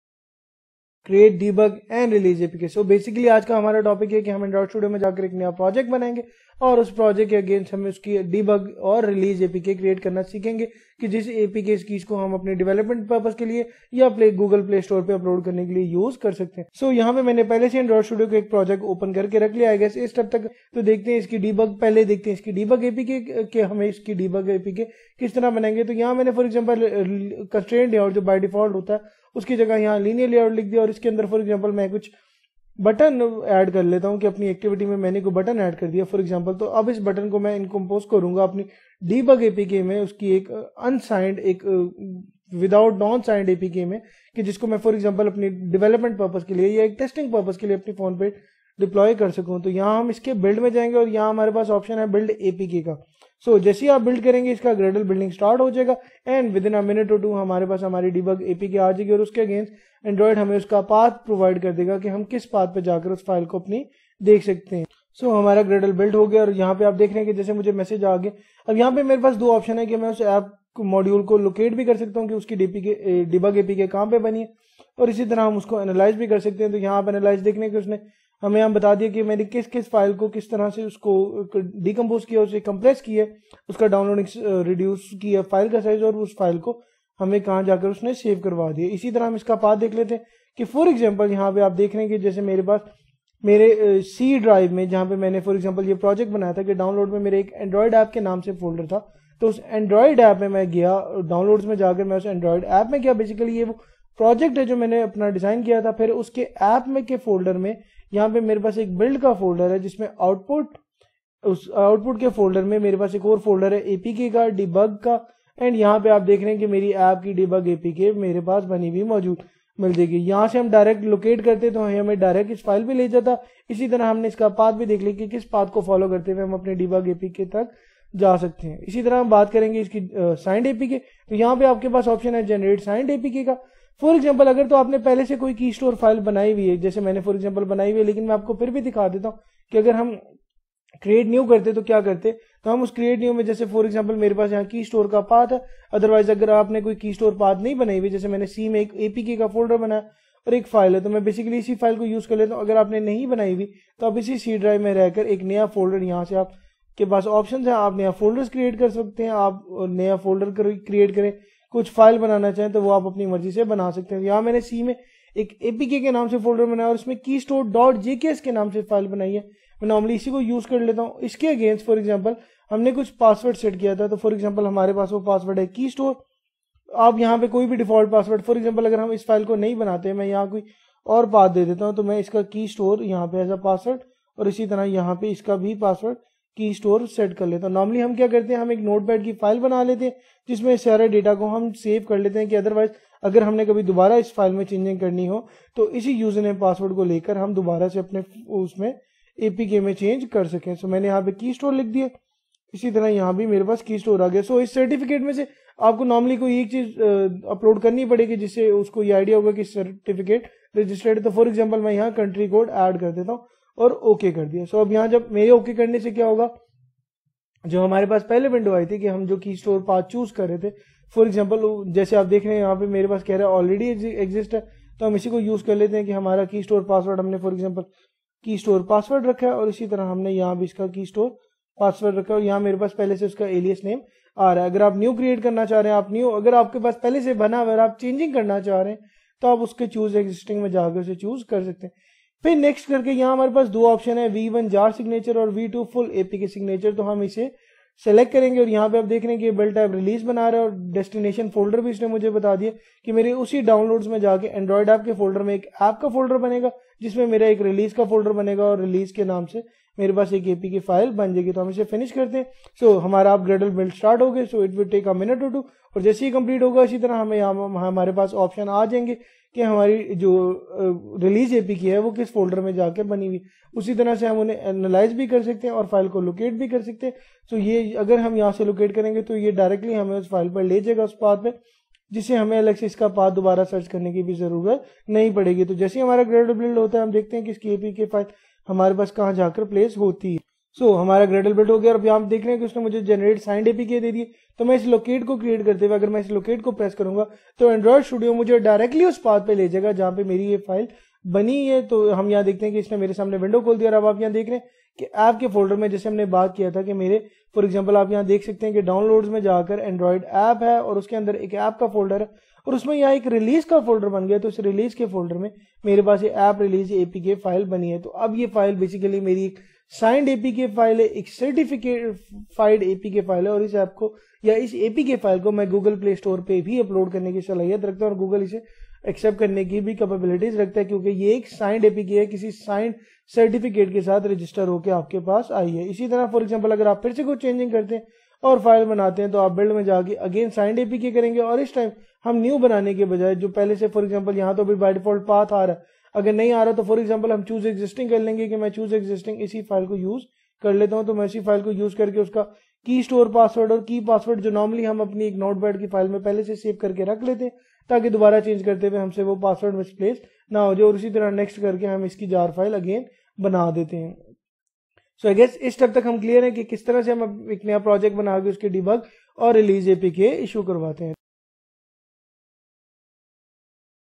Create Debug and Release APK [UR] Module 1 Lecture 18 ZE - 237
Course: "Android App Customization"
Medium of Instruction: Urdu